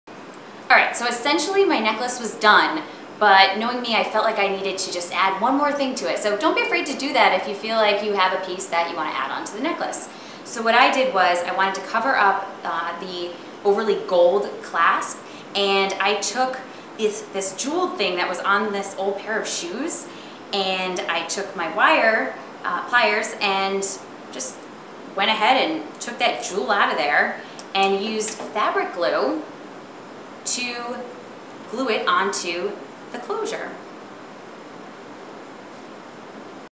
web extra diy necklace
Take your DIY statement necklace up a notch and we're teaching you how to do that using jewels from an old pair of flats.